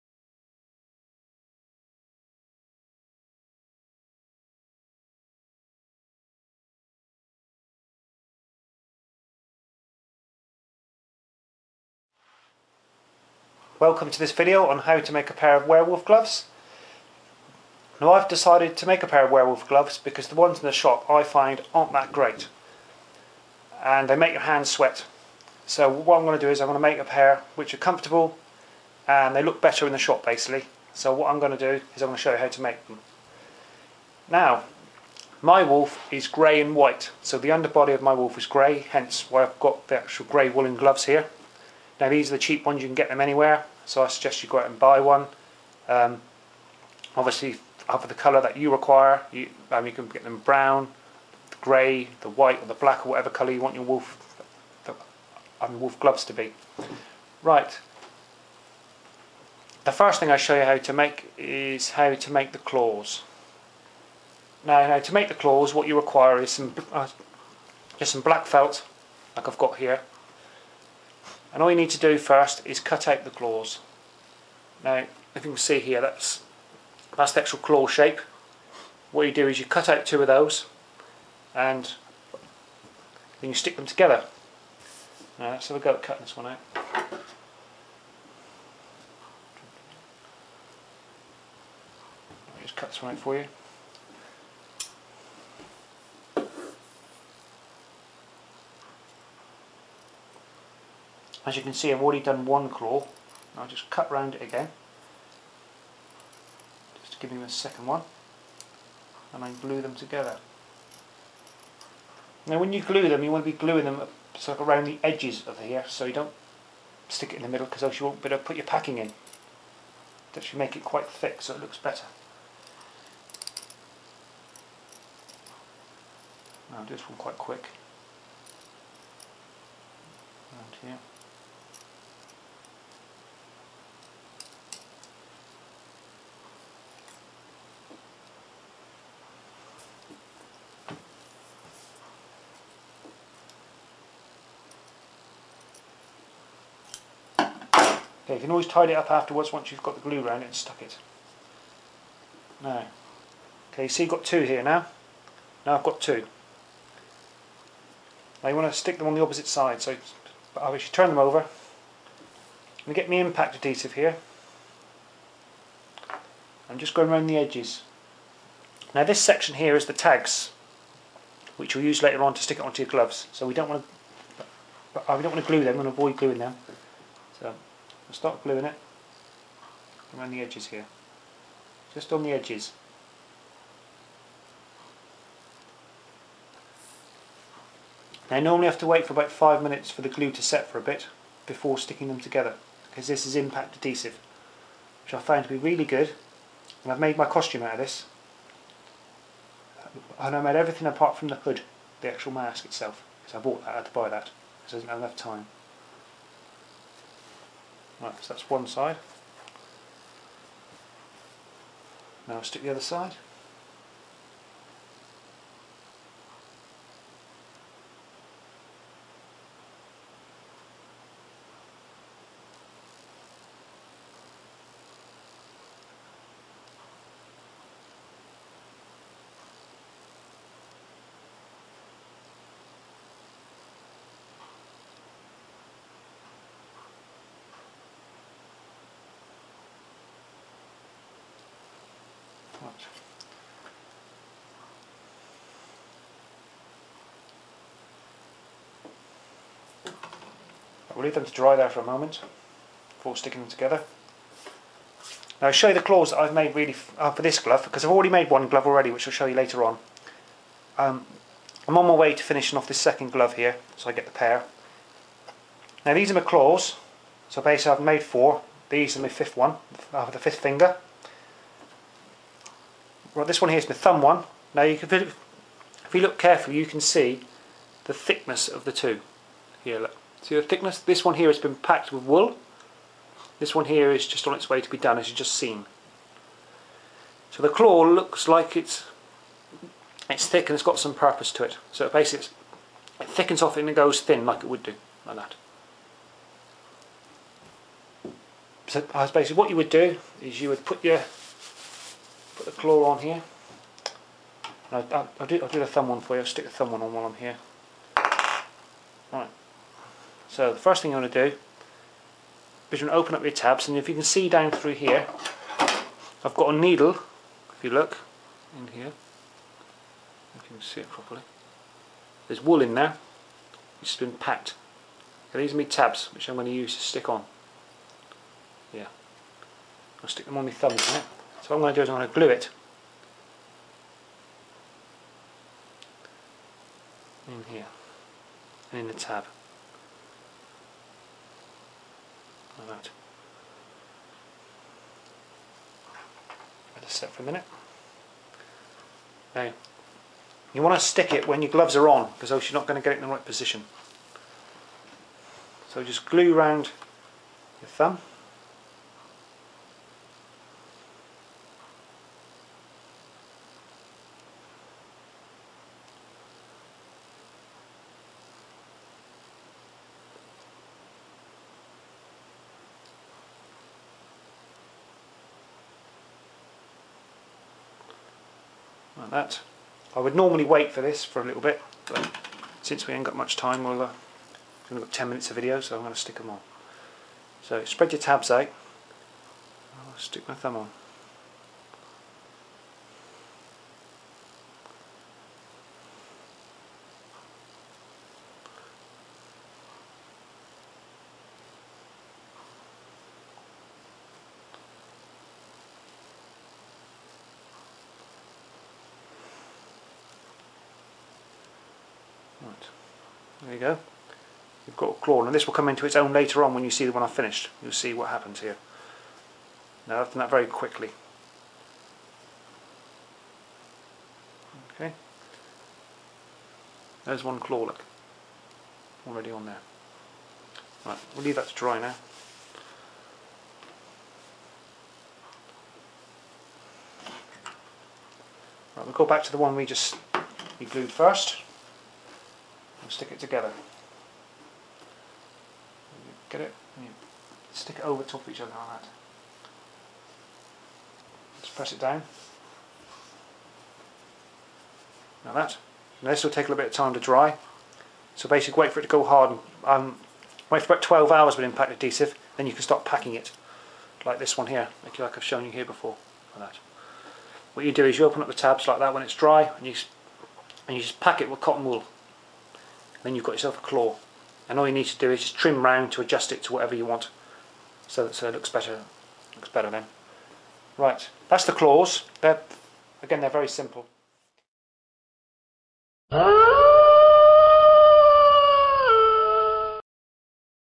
How to make werewolf gloves / hands tutorial Part1. Paul Lugg
I find that the werewolf gloves/hands in the shops do not fit me well and make my hands sweat so I thought I would make my own pair and should you in the process as others my feel the same way.